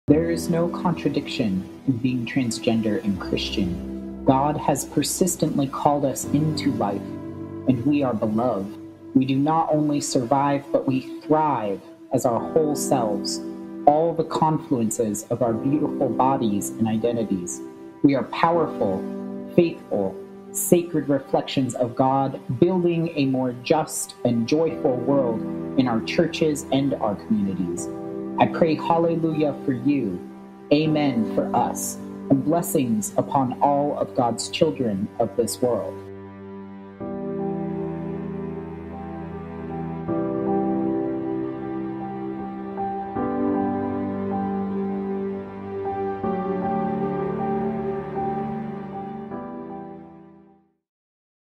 There Is No Contradiction
1 // What would it mean for your church and community to be more just and joyful?
2 // What does it mean for you to thrive as your whole self?

Spoken & Written by Sonny Graves
Footage from VideoBlocks and Pond5

Watch the complete Pride2016 series to hear more reflections from trans and gender nonconforming people